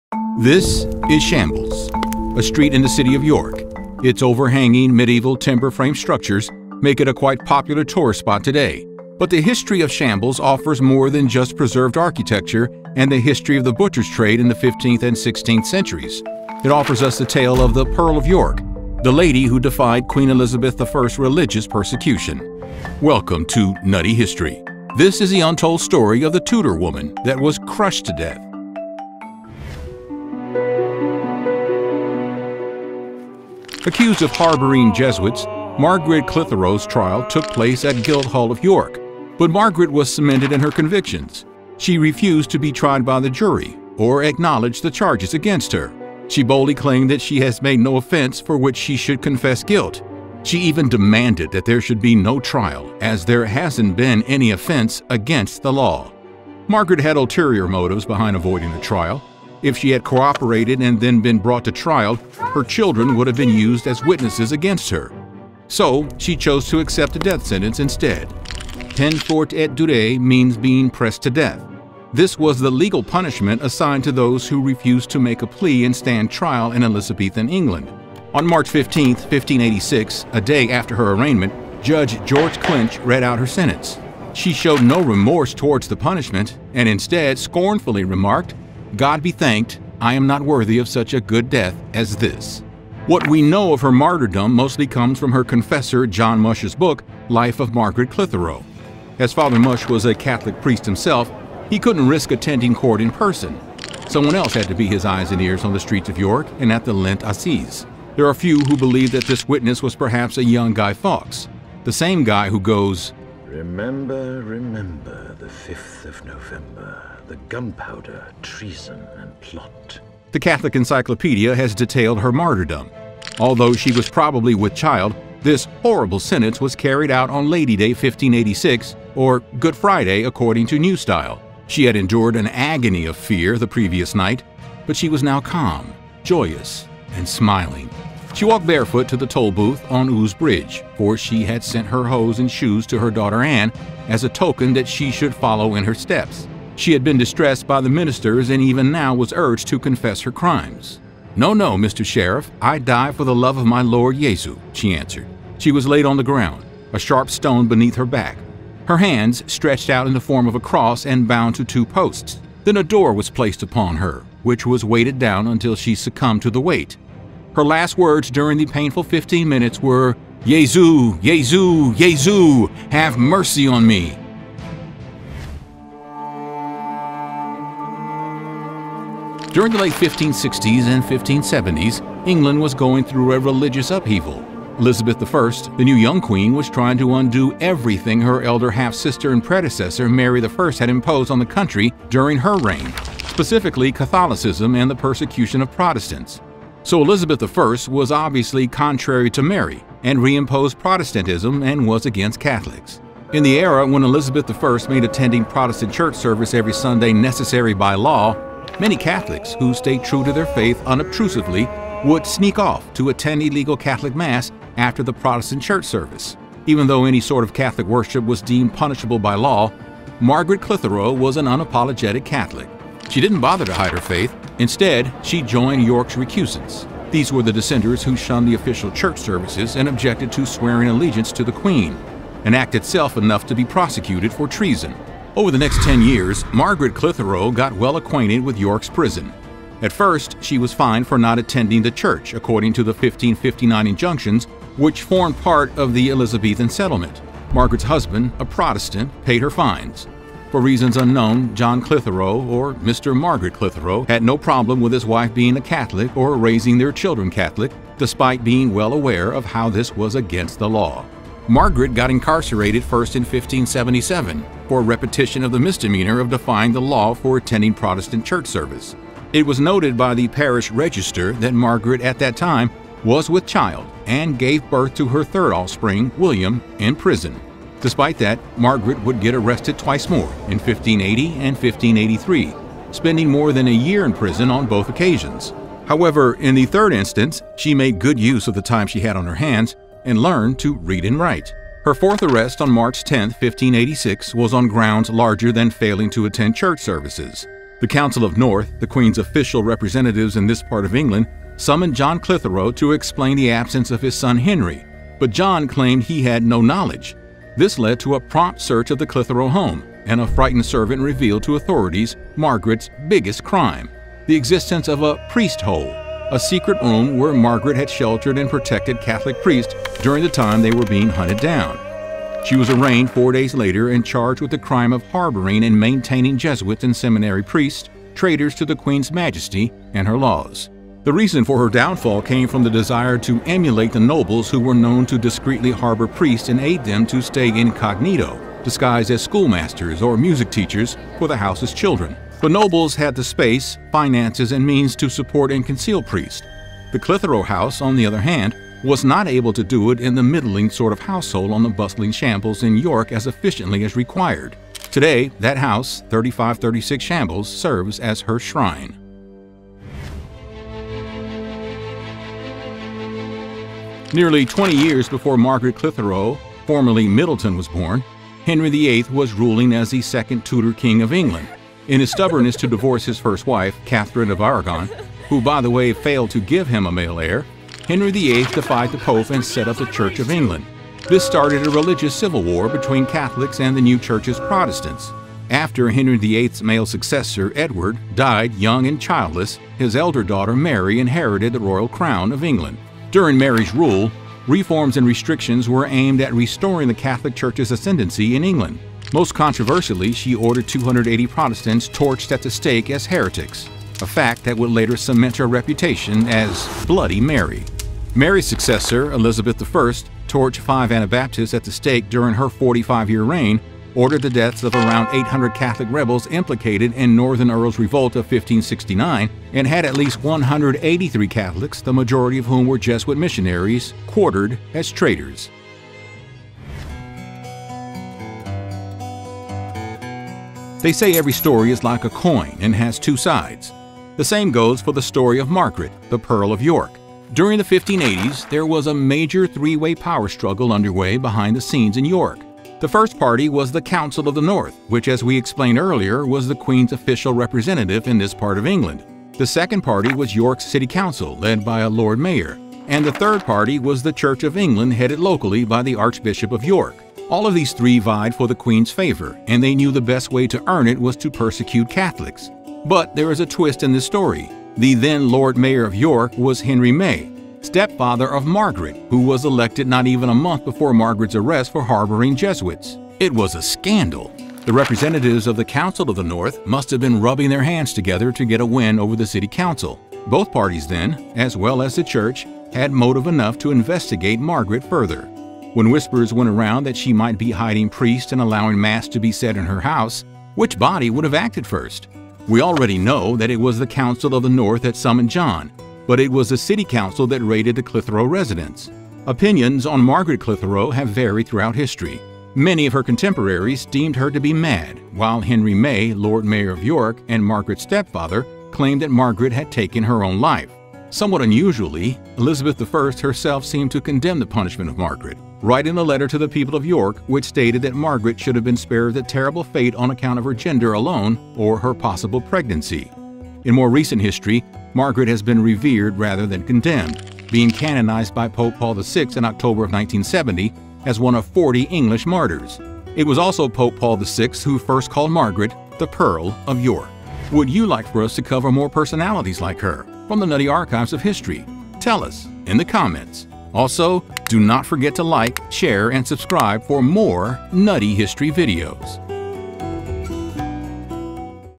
The Untold Story of The Tudor Woman that was "Crushed to Death"
Why and how was this Tudor woman of the Elizabethan Era crushed to death? Was she guilty of her crimes?

Chapter Timestamps
0:00 Intro to mid-roll ads
0:30 The Conviction of Margaret Clitherow
2:56 An Unapologetic Catholic
6:01 England Divided in Two
7:07 The Politics of York

More Content to Binge Watch 🥜